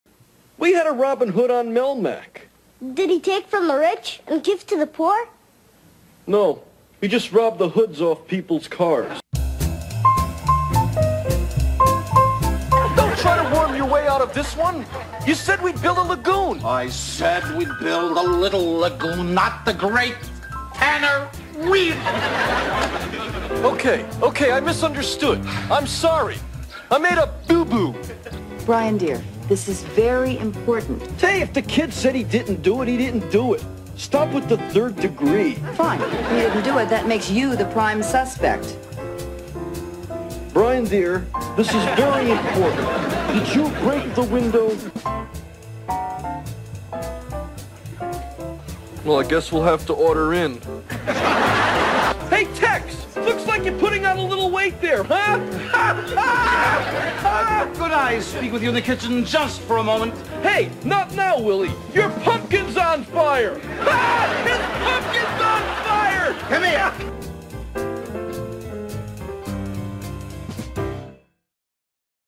Remembering Alf
Mihaly "Michu" Meszaros, the actor who played "Alf," has died at 76-years old.  He was only 33-inches tall and coined "The Smallest Man on Earth."